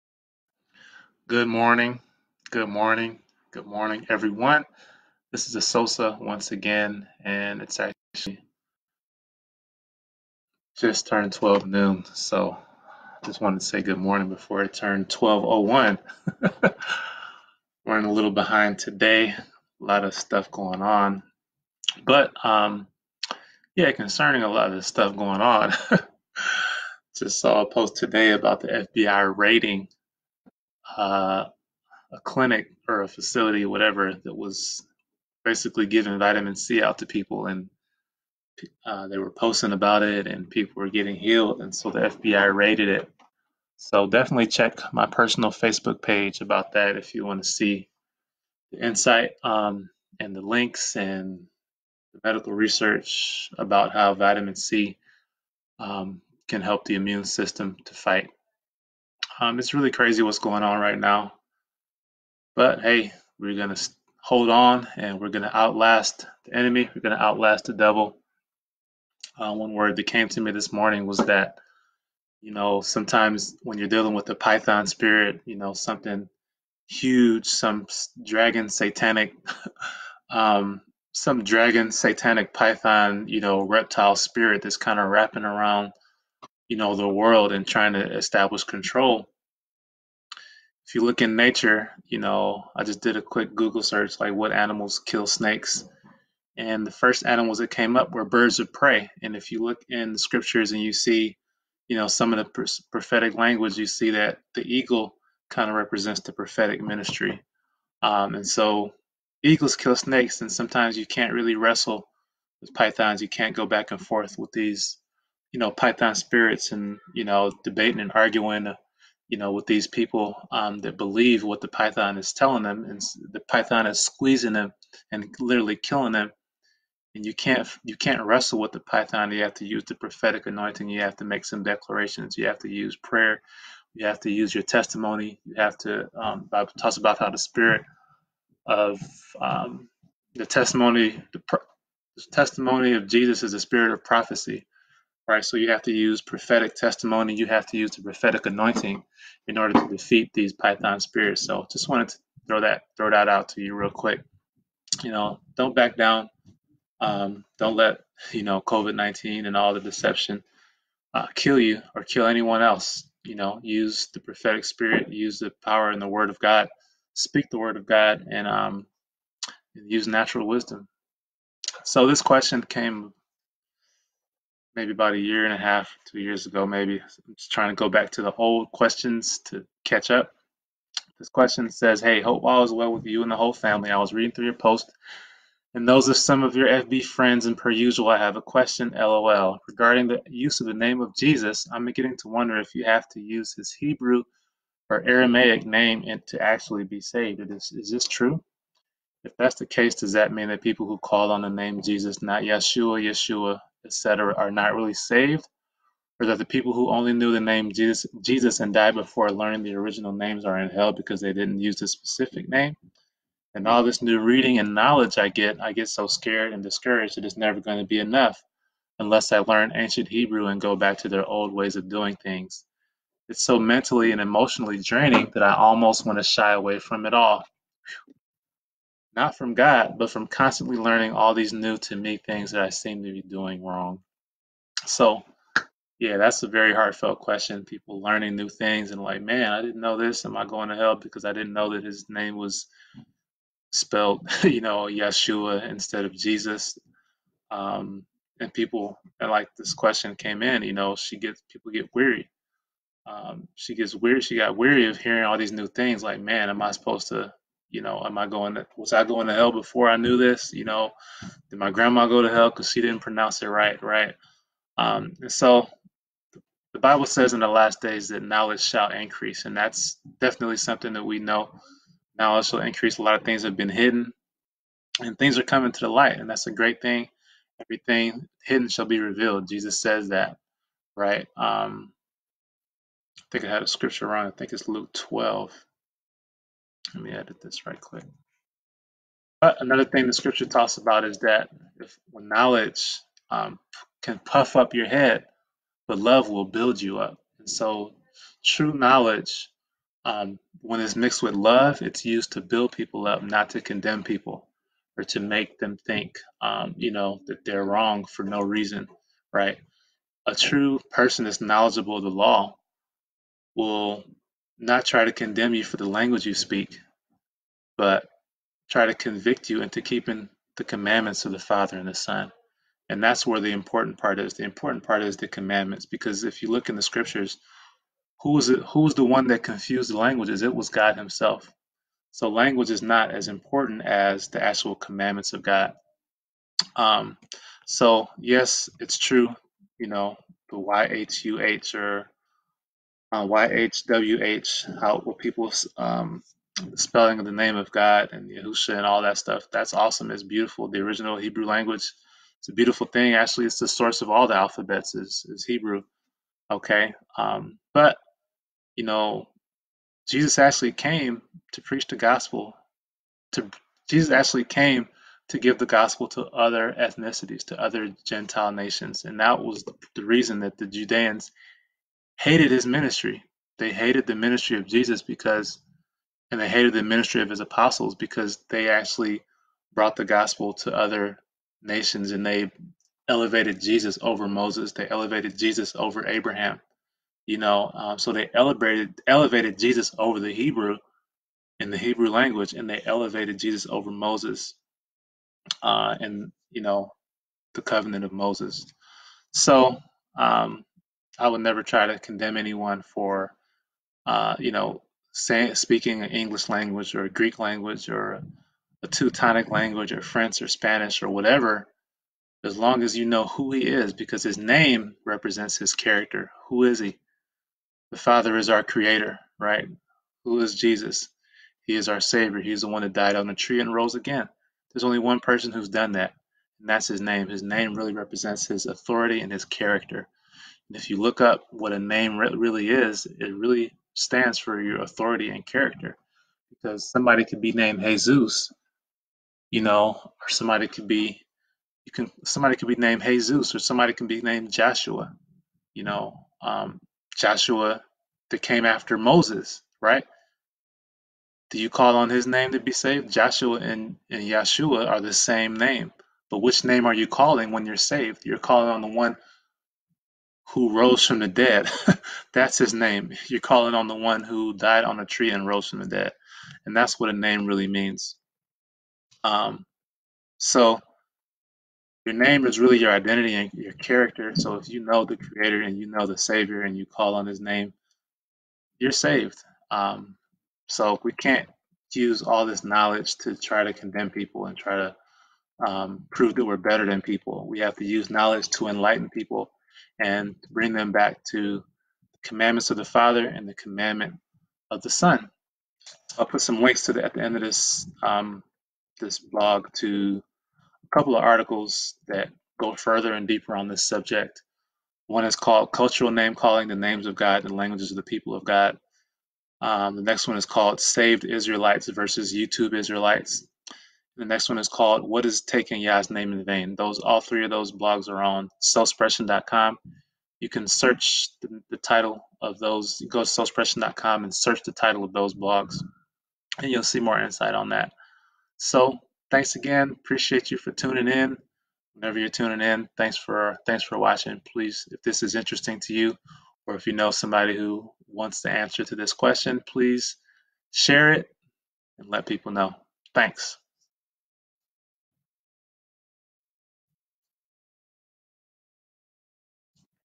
DO I HAVE TO SPEAK HEBREW TO BE SAVED
TRUTH OVER LIES DAILY

Q: Hey! Hope all is well with you and the whole family! I was reading through your posts and those of some of your FB friends and per usual, I have a question. LOL Regarding the use of the name Jesus, I'm beginning to wonder if you have to use is Hebrew/Aramaic name to "actually" be saved. Is this true? If that's the case, does that mean that people who call on the name Jesus, not Yashua, Yeshua, etc are not really saved or that people who only knew the name Jesus and died before learning the original names are in hell, because they didn't use the specific name? In all this new reading and knowledge I get, I get so scared and discouraged that it's never going to be enough, unless I learn ancient Hebrew and go back to their old ways of doing things. It's so mentally and emotionally draining that I almost want to shy away from it all. Not from God, but from constantly learning all these new (to me) things that I seem to be doing wrong.

A: I know there's a lot of knowledge out there. The Bible says that in the last days "knowledge shall increase..."

Also, every thing that is hidden shall be revealed (Luke 18). However, the bible also says that knowledge puffs up, but love builds up.

Although I do understand the Hebrew pronounciation and spelling of YHUH and Yahusha... I would never condemn or try to strangle anyone from saying or praising Jesus Christ or God the Father.

The name of the Most High and His son is good and reverential.

However, His "Name" really stands for His identity and His character. You have to know who He is and what He's done to really know His "Name."

There are many people who say Yahusha all day but don't know His salvation in their soul nor are they filled with the Holy Ghost (Ruach Hachodesh in Hebrew). There's many that call Him a Greek, or English, or Spanish title/pronunciation/transliteration, and God loves them just as much.

It was never about pronunciation as much as it is about repentance, faith, and obedience to His Commandments.

I'm sorry that you feel drained. It's always good to hunger for knowledge, but just be careful with your sources.

Chew the meat, but spit out the bones! Also it it's good to remember that when He died on the Tree (aka cross) His Name was written in Greek, Latin, and Hebrew (Luke 23:38).

The point is, He came, He died, He rose again for everyone in all languages.

CULTURAL NAME CALLING, THE NAMES OF GOD AND THE LANGUAGES OF THE PEOPLE OF GOD

WHAT IS TAKING YAH'S NAME IN VAIN?